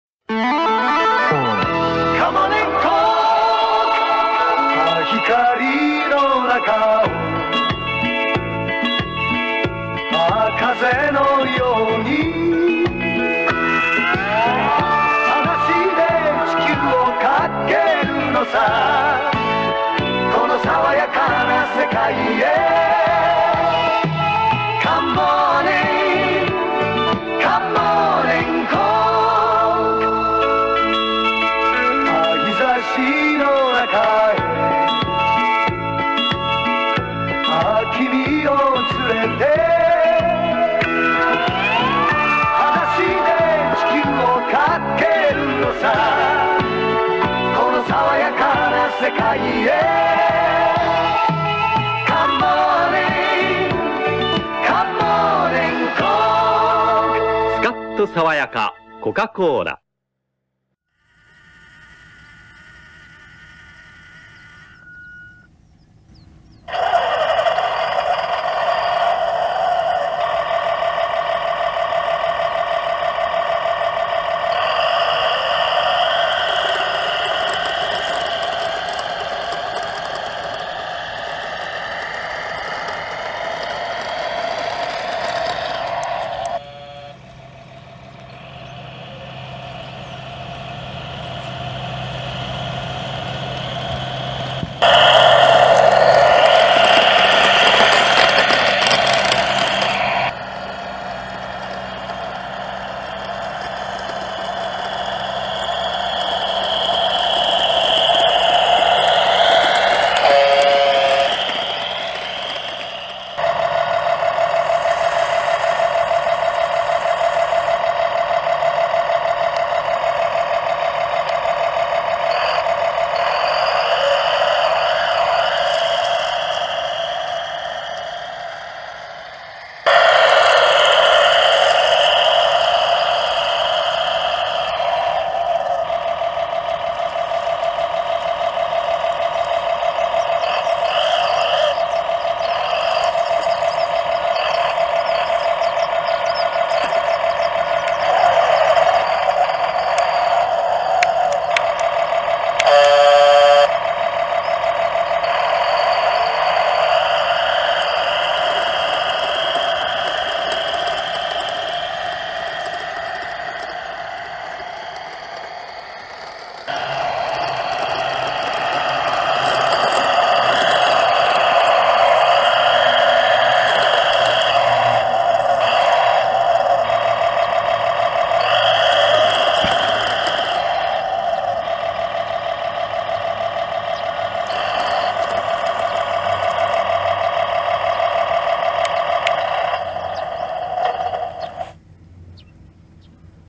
コカ・コーラ ラジコントレーラー改造
Coca-Cola RC Trailer
TX/RX Taranis QX7
DasMikro Programmable Sound Unit for Europe And US Trailer Truck